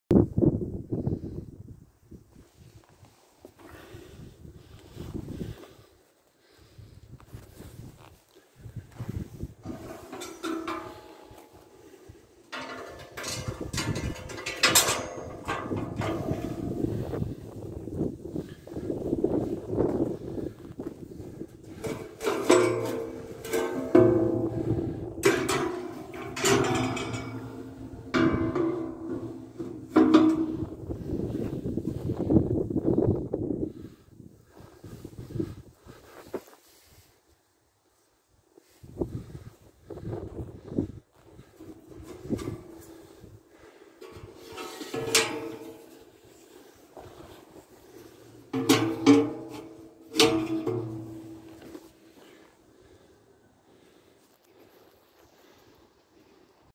Мангал
мангалсвоимируками мангал самодельный,материал остатки,обрезки,уголки(чермет).